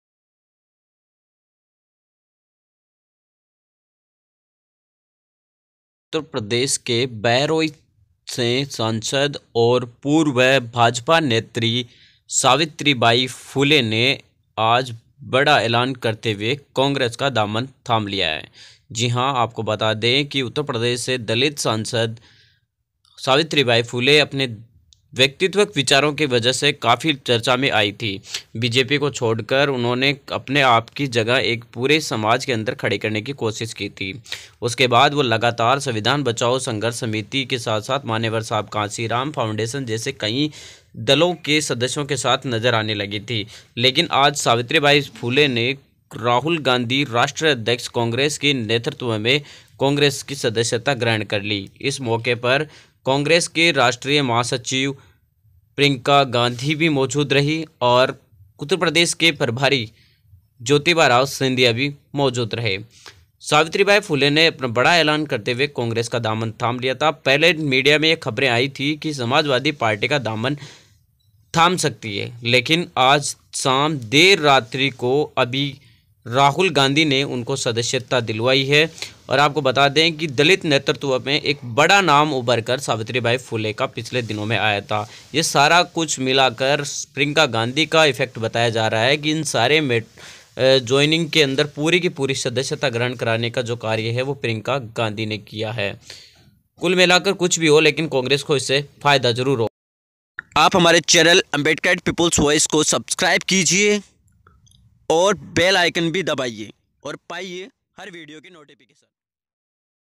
Bahraich MP Savitri Bai Phule join Congress || सांसद सावित्री बाई फूले कांग्रेस में शामिल
Delhi: Bahraich MP Savitri Bai Phule (former BJP leader) and former Fatehpur MP Rakesh Sachan (former SP leader) join Congress in presence of Party President Rahul Gandhi, General Secretary for UP east Priyanka Gandhi Vadra and General Secretary for UP west Jyotiraditya Scindia.